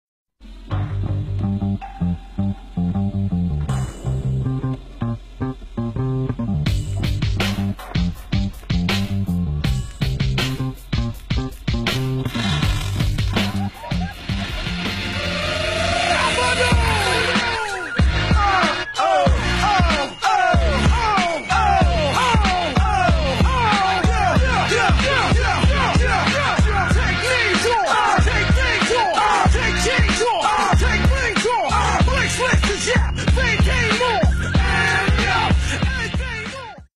Funny Dog playing with fire
That moment when your dog try to protect you and you will be in danger.LOL